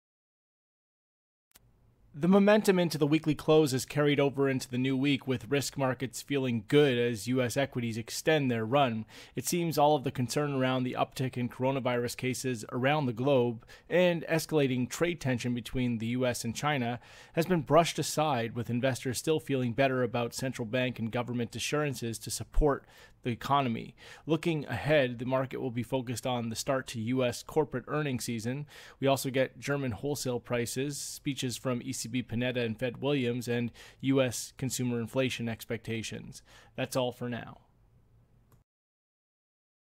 Positive momentum carries over into new week
The momentum into the weekly close has carried over into the new week, with risk markets feeling good and US equities extending their run.